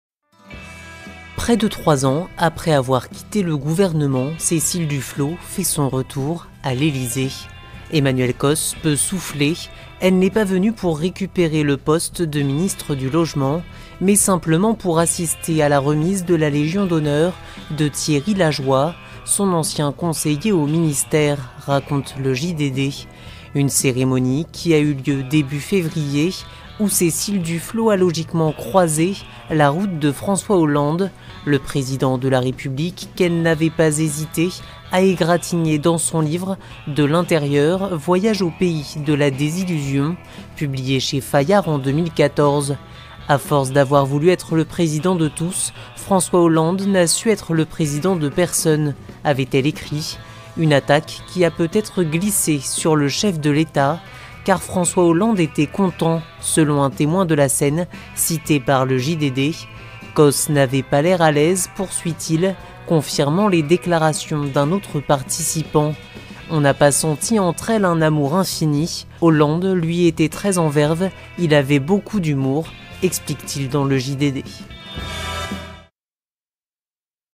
Cécile Duflot : les coulisses de son retour à l'Elysée
Près de trois ans après avoir quitté le gouvernement, Cécile Duflot fait son retour à l'Elysée. Emmanuelle Cosse peut souffler, elle n'est pas venue pour récupérer le poste de ministre du Logement, mais simplement pour assister à la remise de la Légion d'honneur de Thierry Lajoie, son ancien conseiller au ministère raconte le JDD. Une cérémonie qui a eu lieu début février où Cécile Duflot a logiquement croisé la route de François Hollande, le président de la République qu'elle n'avait pas hésité à égratigner dans son livre "De l'intérieur, voyage au pays de la désillusion", publié chez Fayard en 2014. "À force d'avoir voulu être le président de tous, François Hollande n'a su être le président de personne" , avait-elle écrit. Une attaque qui a peut-être glissé sur le chef de l'Etat, car "François Hollande était content" selon un témoin de la scène cité par le JDD. "Cosse n'avait pas l'air à l'aise" poursuit-il, confirmant les déclarations d'un autre participant. "On n'a pas senti entre elles un amour infini. Hollande, lui, était très en verve, il avait beaucoup d'humour" , explique-t-il dans le JDD.